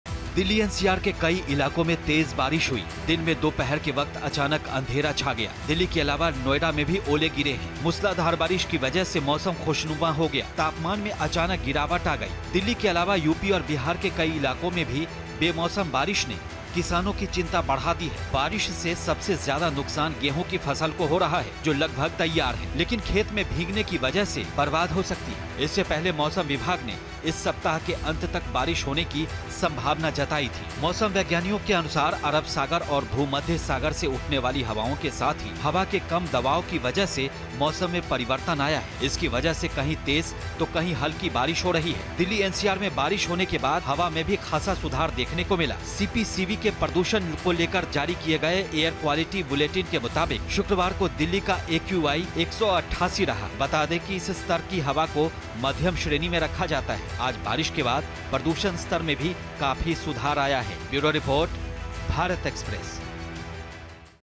Weather Update: दिल्ली-NCR की सड़कों पर बिछ गए ओले, बारिश ने बढ़ाई किसानों की चिंता
दिल्ली एनसीआर के कई इलाकों में तेज बारिश हुई.दिन में दोपहर के वक्त अचानक अंधेरा छा गया.दिल्ली के अलावा नोएडा में ओले गिरे हैं.मुसलाधार बारिश की वजह से मौसम खुशनुमा हो गया.तापमान में अचानक गिरावट आ आई है.दिल्ली के अलावा यूपी और बिहार के कई इलाकों में भी बेमौसम बारिश ने किसानों की चिंता बढ़ा दी हैं.बारिश से सबसे ज्यादा नुकसान गेंहू की फसल को हो रहा है. जो लगभग तैयार है लेकिन खेत में भींगने की वजह से बर्बाद हो सकता है.

Social Media -